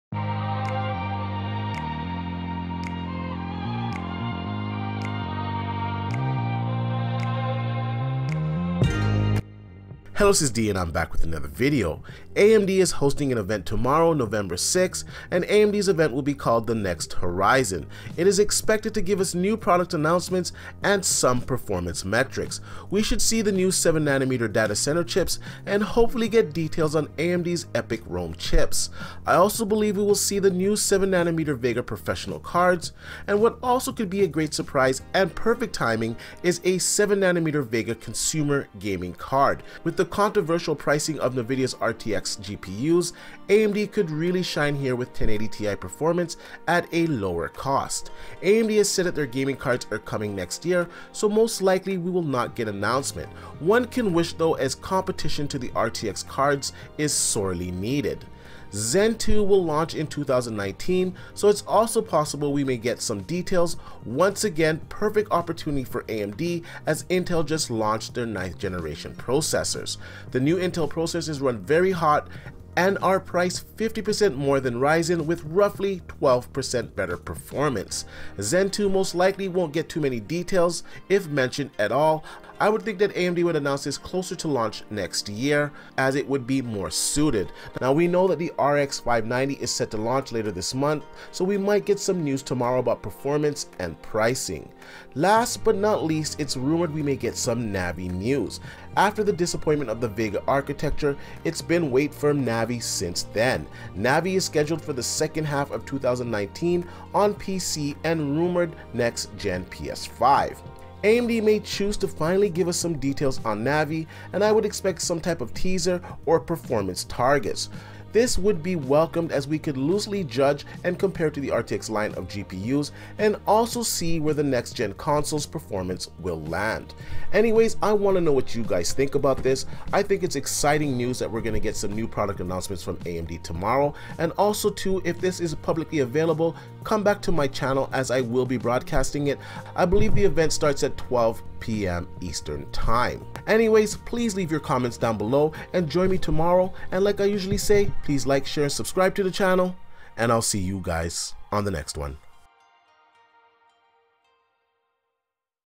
AMD To Host NEXT Horizon Event Nov 6th
AMD Next Horizon Event scheduled for NOV 6th! Lets talk about what AMD may show..